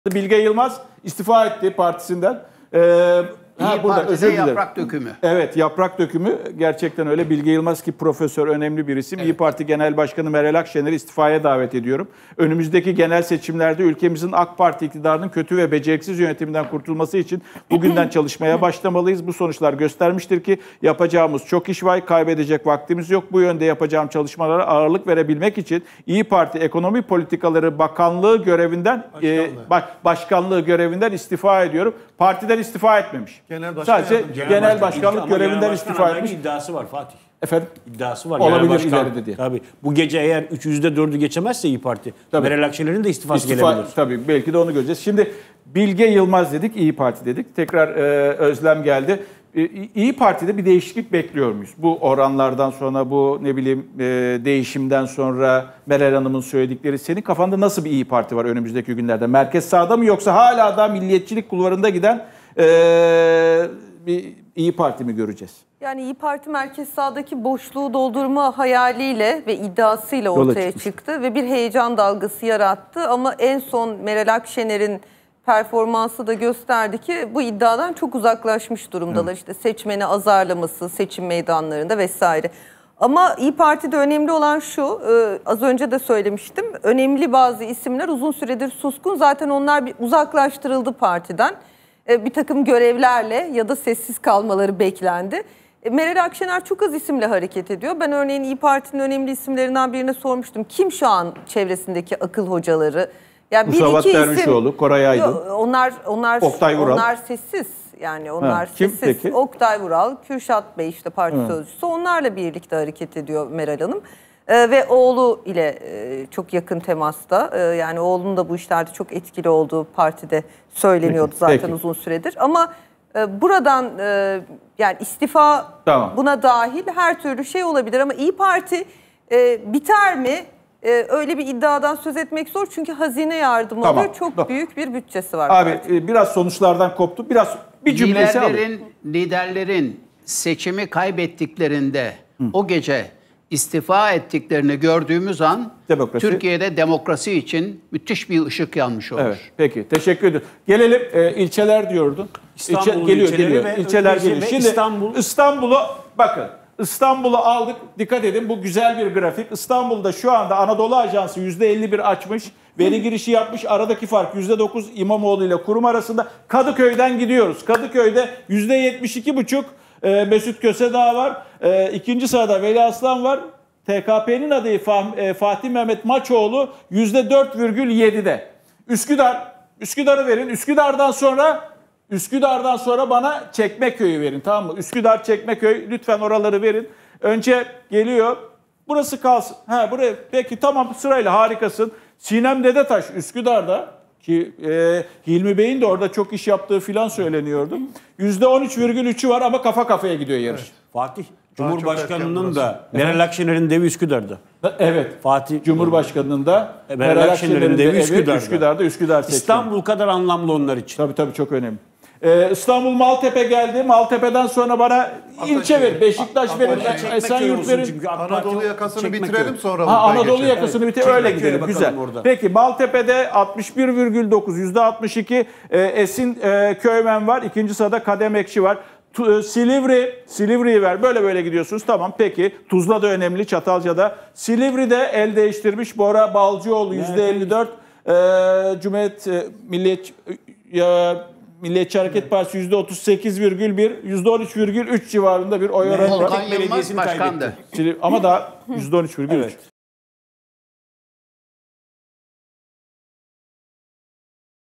İYİ Parti Hezimet Yaşıyor! Bilge Yılmaz Meral Akşener'i İstifaya Davet Etti
Digiturk: 60
D-smart: 93
Turkcell TV Plus: 69
Tivibu: 69
Türksat Kablo TV: 76